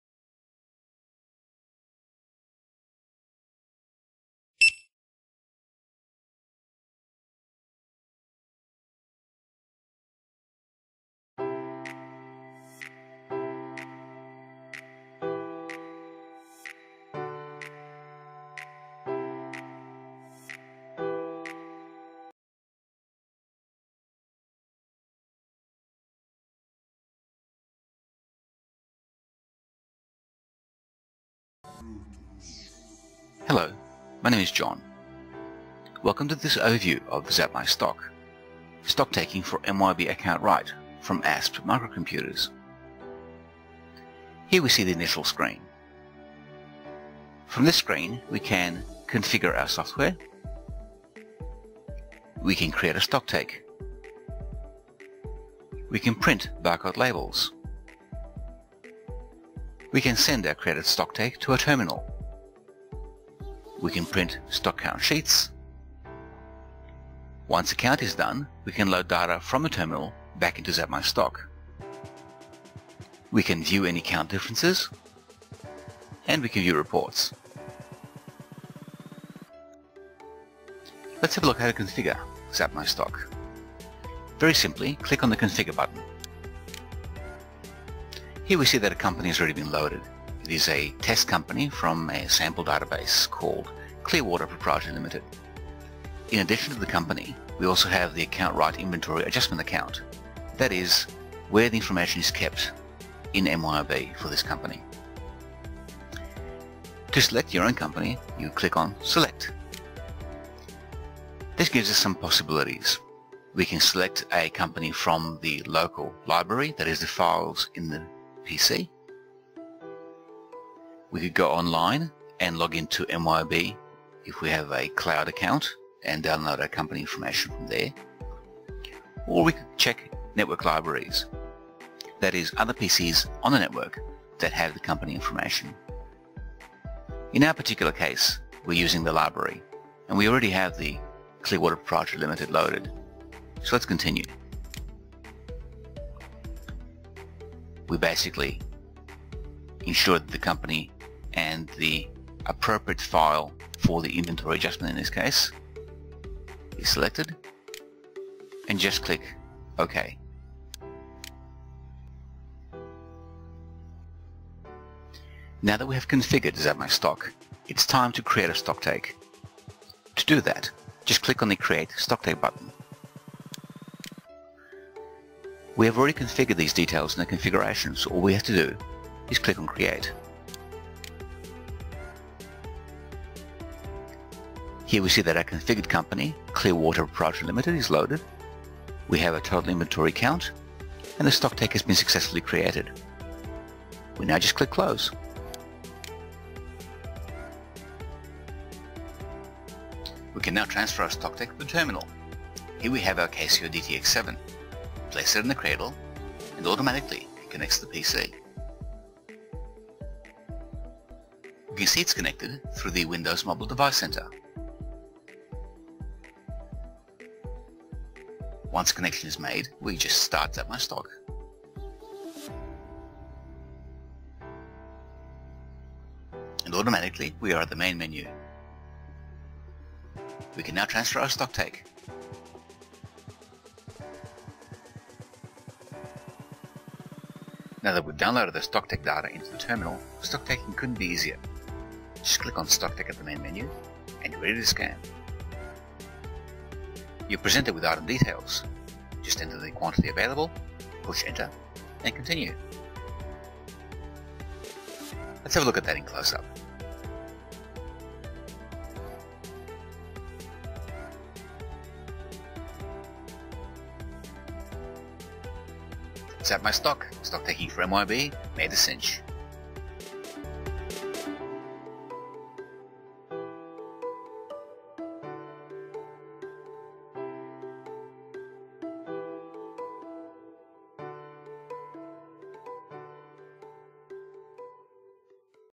Conducting a Stocktake
Do all your staff suddenly apply for leave when you mention stocktake?

That's because you're not using zapMYstock. Conducting a stocktake can be difficult and time consuming. We decided that there had to be a better way, then we set out to design it. The result is zapMYstock - a low cost, simple to use stocktake add-on that integrates with your existing MYOB AccountRight Live Plus or Premier to reduce the burden of stocktaking.

[re-upload]

Address:
ASP Microcomputers
14 Business Park Drive
Notting Hill, VIC 3168
Australia

ABN:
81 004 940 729